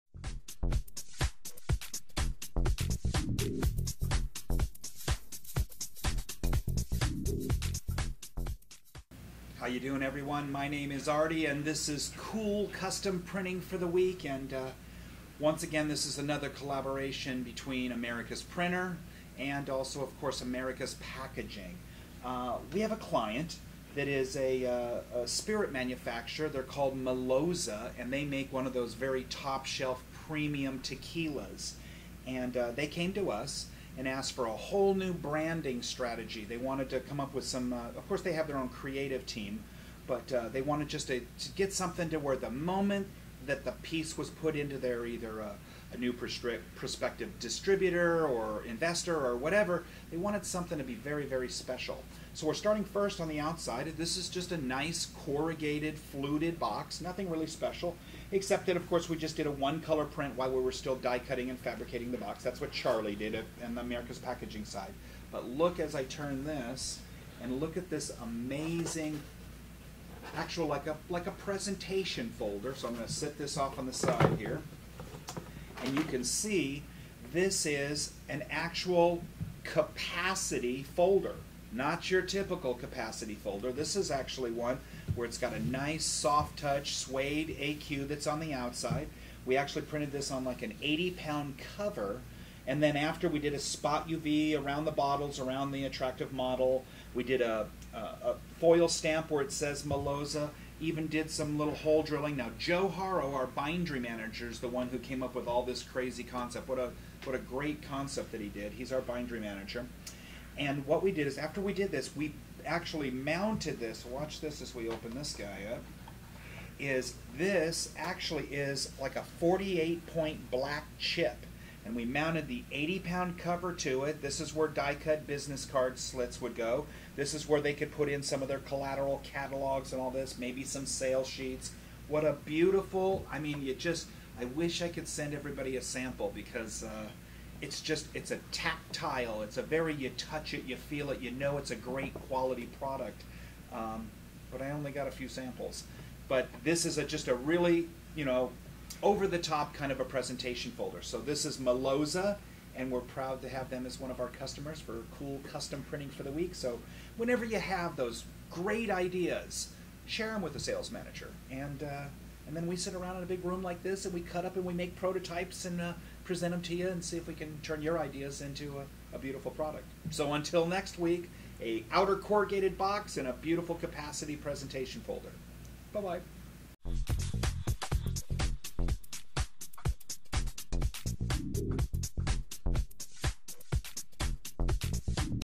Custom investor kit box with fold out capacity pocket. Cool Custom Printing of the week
This was made for a premium tequila company.  This folder box has it all, foil, spot UV suede AQ coating and an elastic band strap.  Really a remarkable custom presentation box.